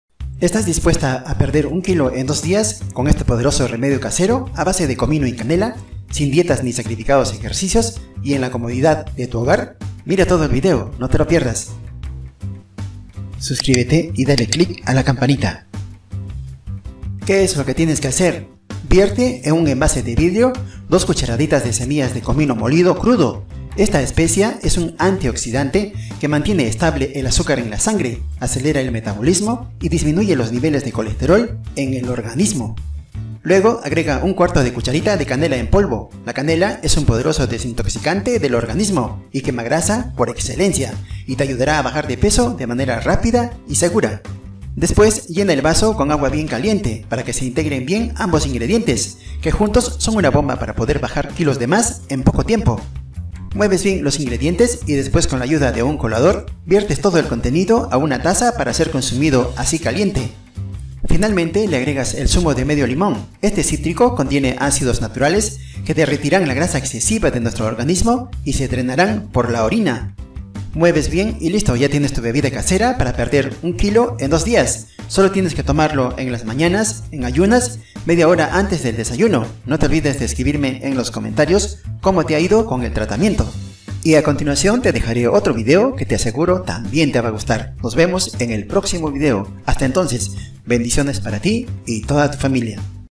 Pierde 1 Kilo en 2 Días con Comino y Canela, la Mejor Mezcla para Adelgazar
Estas dispuesta a perder un kilo en dos días con este poderoso remedio casero a base de comino y canela, sin dietas ni sacrificados ejercicios y en la comodidad de tu hogar, mira todo el video, no te lo pierdas.

¡Advertencia!
Los contenidos de esta publicación se redactan solo con fines informativos. En ningún momento pueden servir para facilitar diagnósticos o sustituir la labor de un profesional. Le recomendamos que contacte con su especialista de confianza.